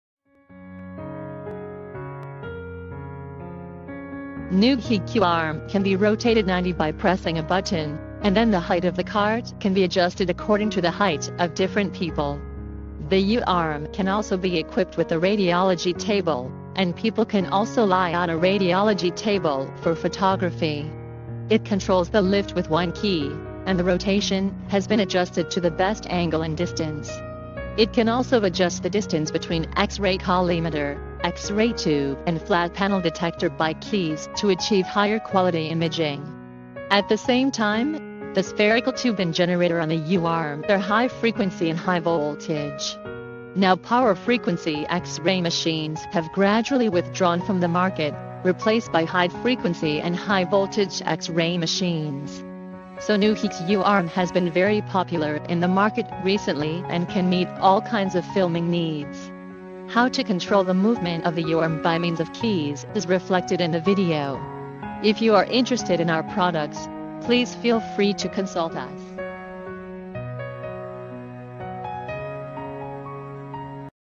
What button does the movement of the U-arm
Newheek U-arm can be operated by different buttons to achieve different shooting methods.Other X-ray machine accessories you can find in Newheek.please click the web